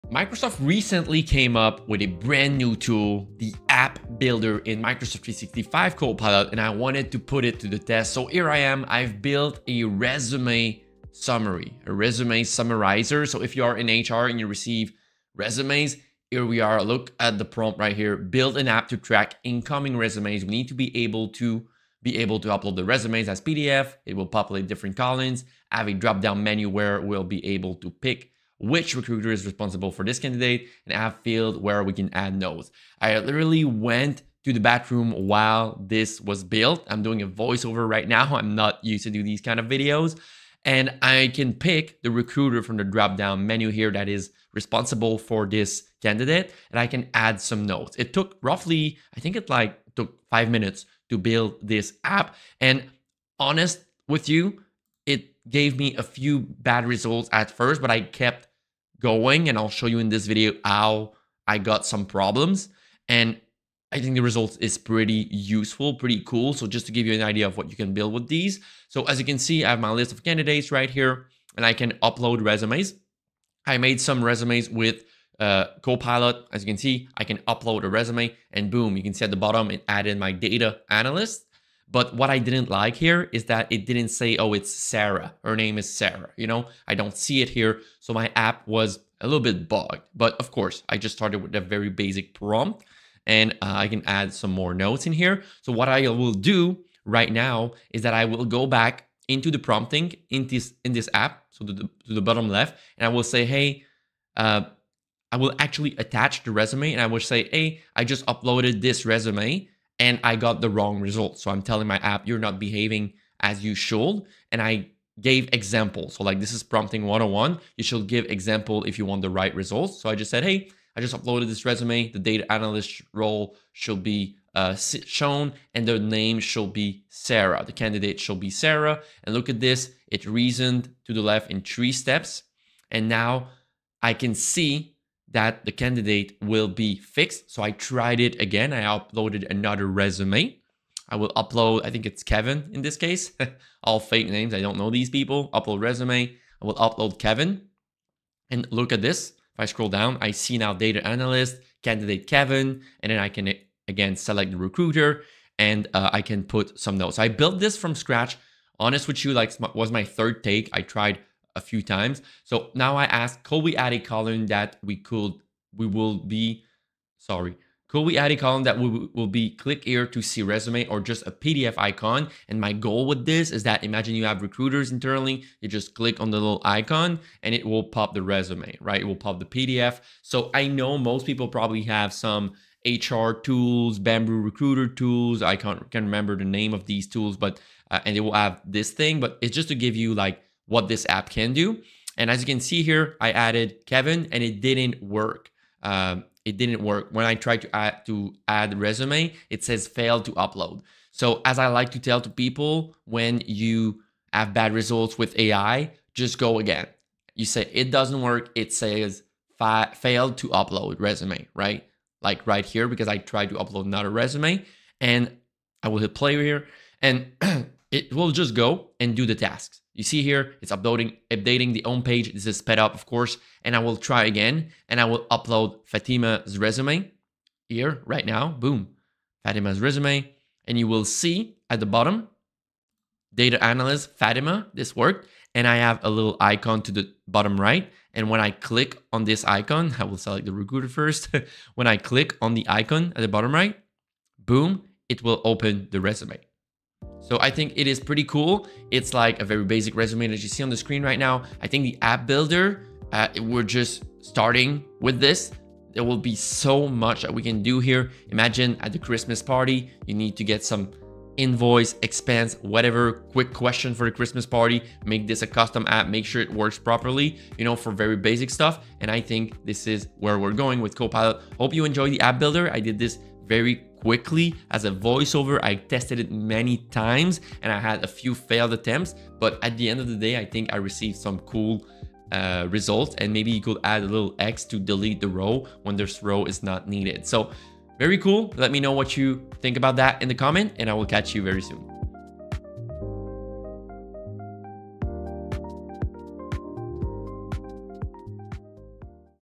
I Tested the NEW Copilot App Builder Agent So You Don't Have To
In this video I show you how I built a HR resumé tracker with the brand new Copilot App Builder agent. It is available in Microsoft 365 Copilot in the Frontier firm, it should come out "Microsoft Soon". Building app with Microsoft 365 Copilot does not require any code.

Corporate Training, want to train your whole team on the Microsoft Copilot Topic?